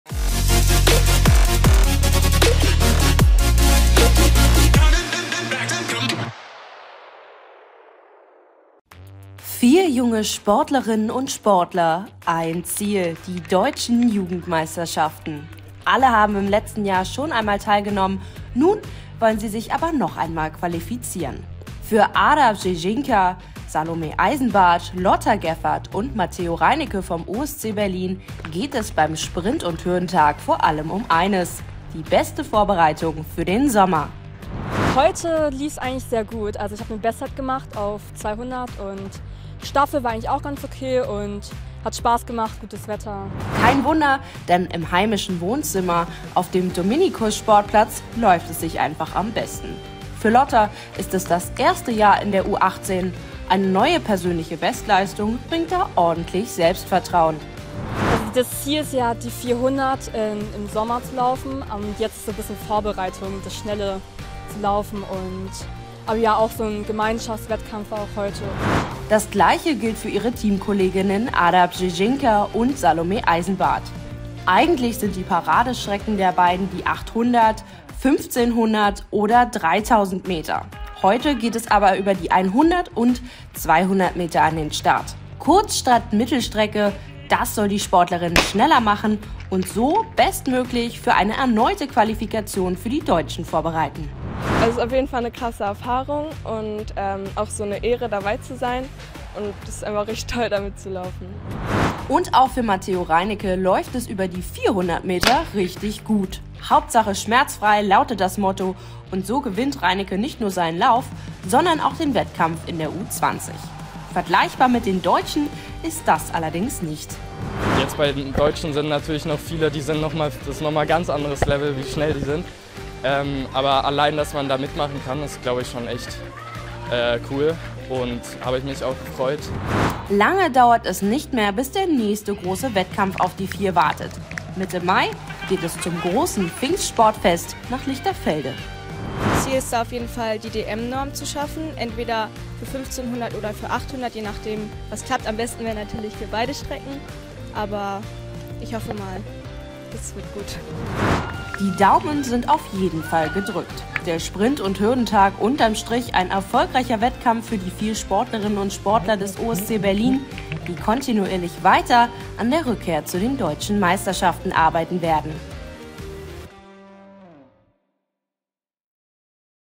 Diese JUNGEN TALENTE des OSC BERLIN wollen zu den DEUTSCHEN MEISTERSCHAFTEN! I Sprint- und Hürdentag
4 junge Sportlerinnen und Sportler - 1 Ziel. Die Deutschen Meisterschaften. Alle haben im letzten Jahr schon einmal teilgenommen, nun wollen sich alle noch einmal qualifizieren. Für Ada Brzezinka, Salome Eisenbarth, Lotta Geffert und Mattheo Reineke vom OSC Berlin geht es beim Sprint- und Hürdentag vor allem um eines: die beste Vorbereitung für den Sommer.